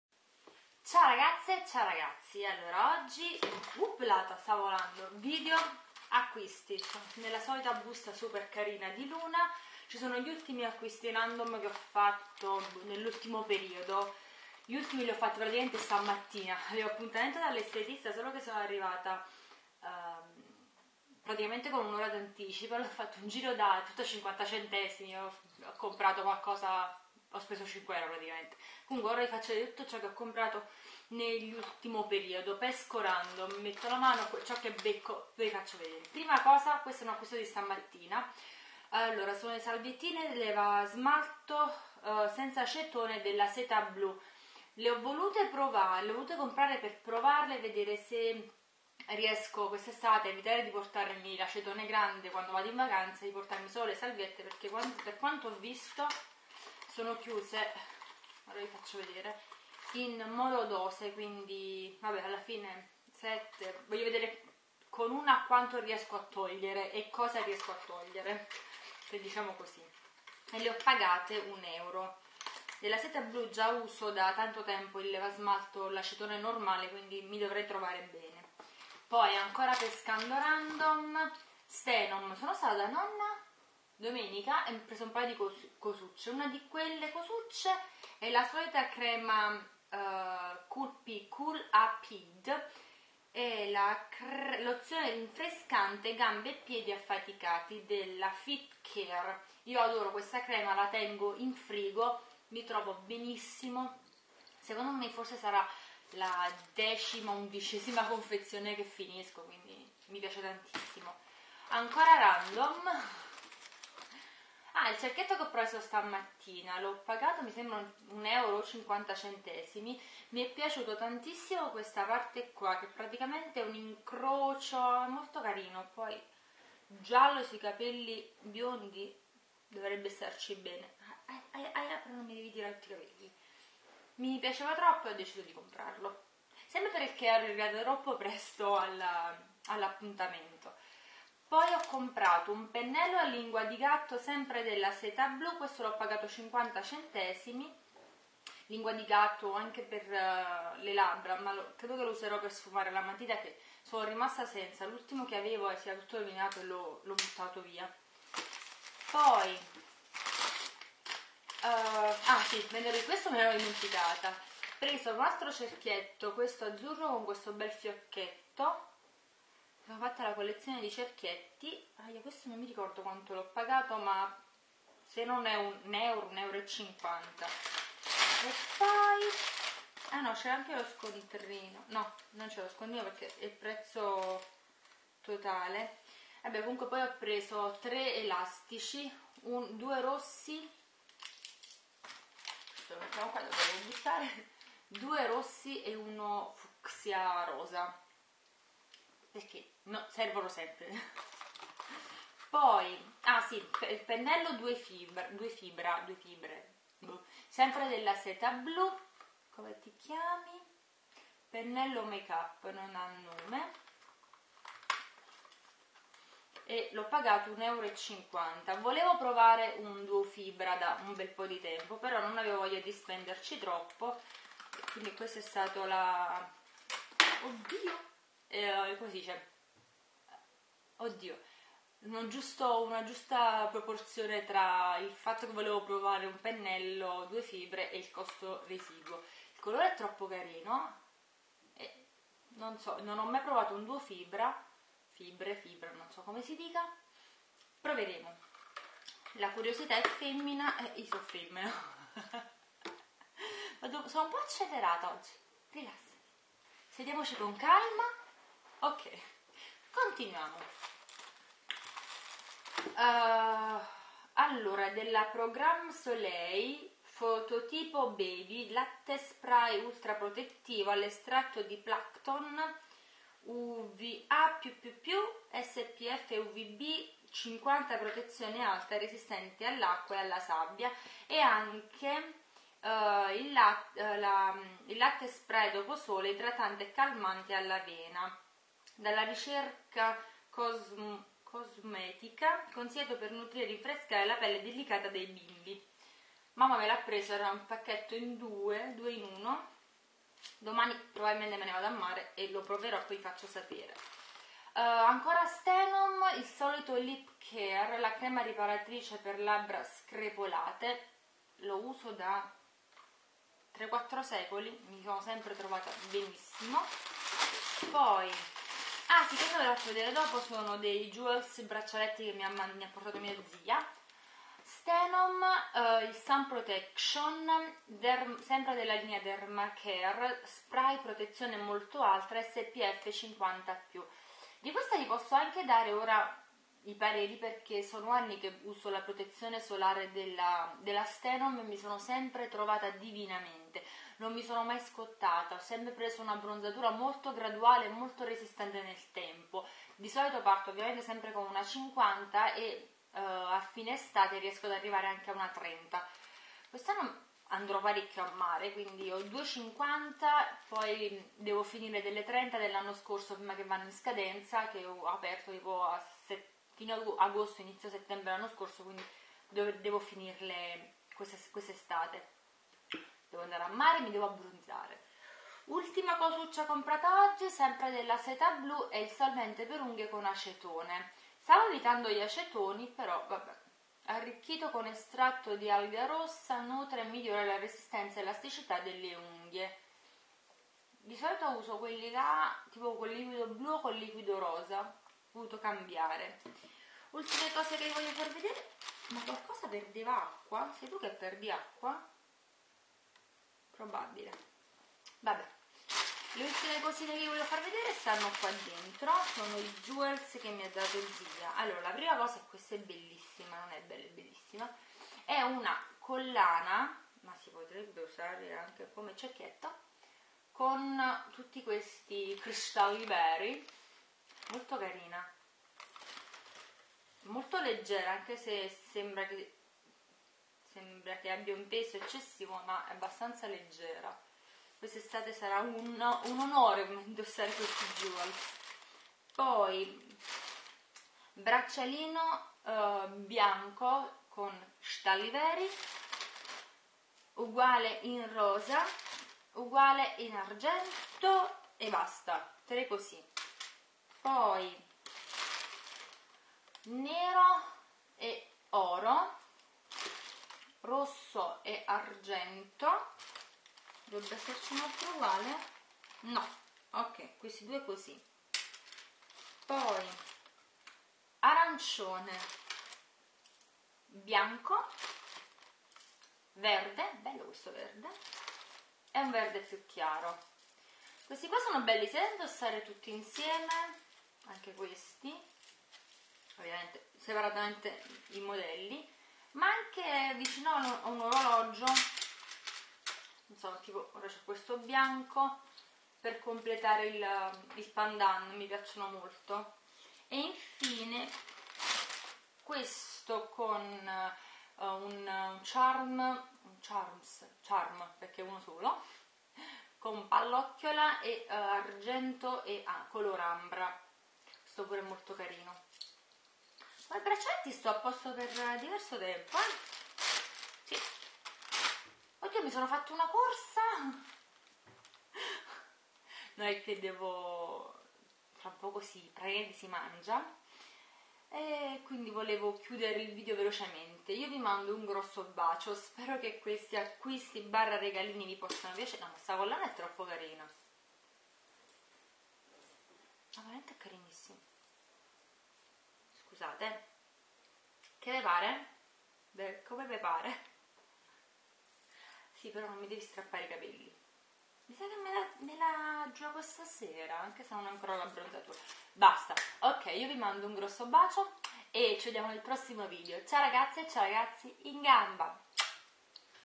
Acquisti e regali random
Ciao
altro video i cui protagonisti sono gli acquisti.
Ecco la lista:
Setablu salviette leva smalto profumate senza acetone 7 pezzi
Stanhome feet care cool a ped lozione rinfrescante gambe e piedi affaticati
Cerchietto giallo
Setablu pennello trucco
Cerchietto azzurro con fiocco
Elastici rossi
Elastico rosa
Setablu pennello makeup
Programme solaire dermotipo baby latte spray ultra protettivo all’estratto di plancton SPF UVB UVA+++ 50 protezione alta
Programme solaire dermotipo baby latte spray doposole idratante e calmante all’avena
Stanhome derma care lip care crema riparatrice labbra screpolate
Stanhome derma care sun protect spray protezione molto alta SPF 50+
Setablu nail polish remover solvente per unghie con acetone
Collana
Bracciali
Spero di avervi tenuto compagnia
Un bacio